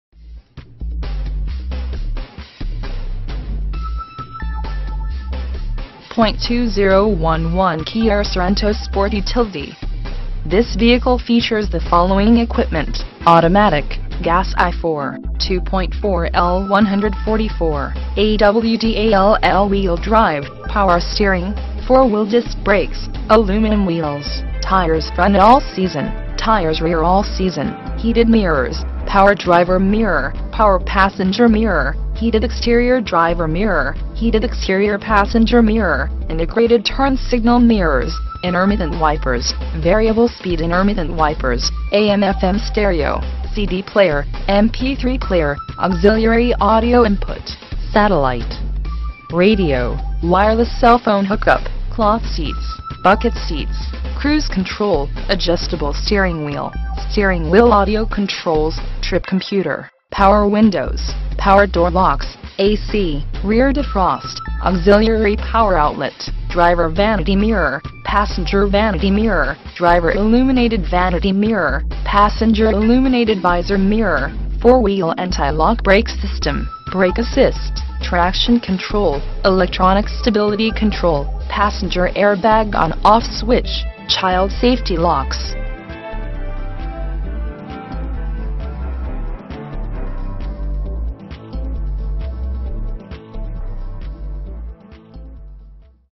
2011 Kia Sorento - Classic Dealer Group
For Sale: 2011 Kia Sorento
VIN: 5XYKTCA14BG013646
Engine: Gas I4 2.4L/144
Drivetrain: AWD
Transmission: Automatic
Mileage: 16,337
Color: Spicy Red (exterior) Beige (interior)

Features:
2011 Kia Sorento Sport Utility.